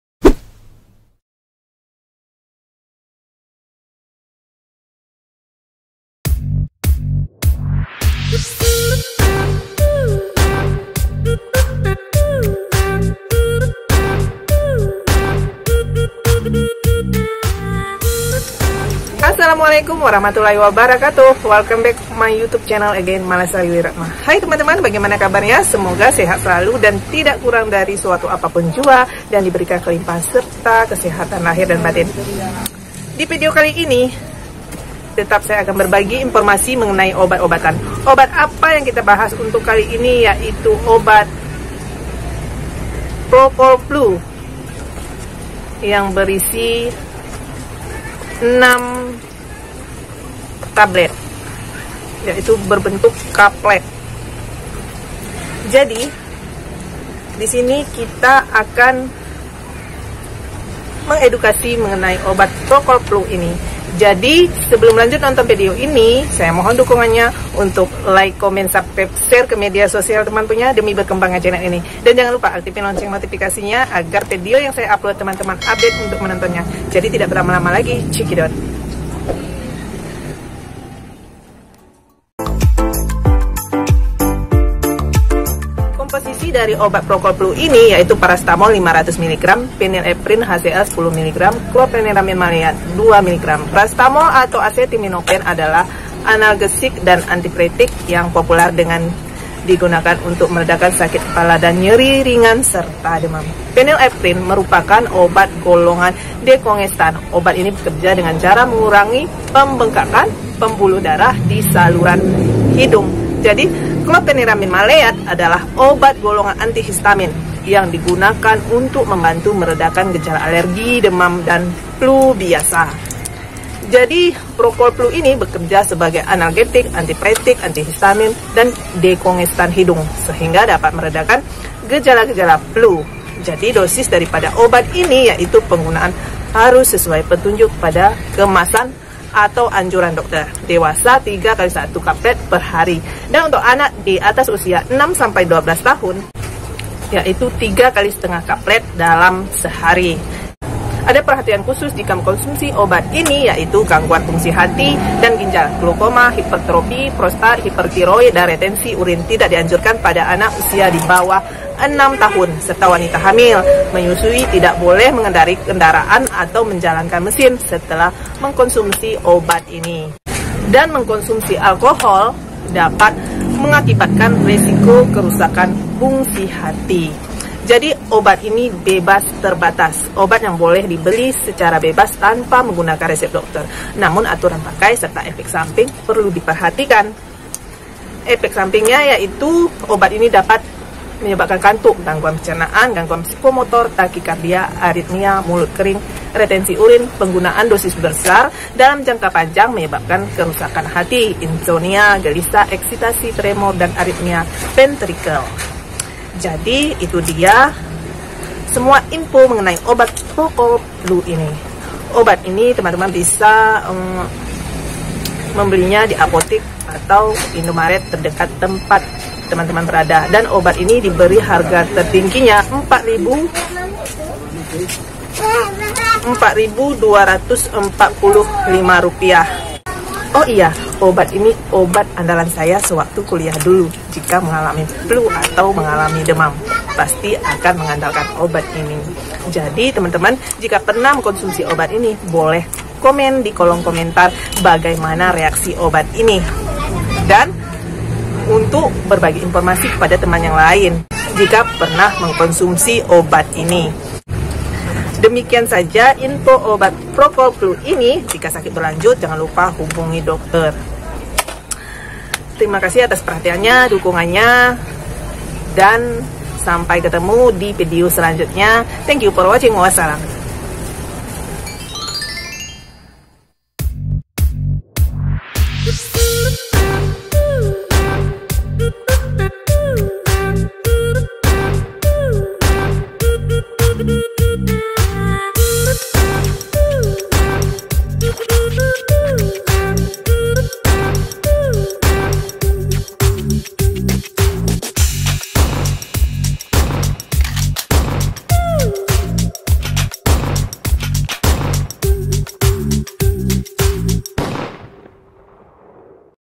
Obat DEMAM Dan FLU
Komposisi
Parasetamol500 mg,
phenylephrine HCl 10 mg,
chlorpheniramine maleat 2 mg.

Dosis

Dewasa
3 x 1 kaplet/hari.

Anak 6-12 tahun:
3 x ½ kaplet/hari.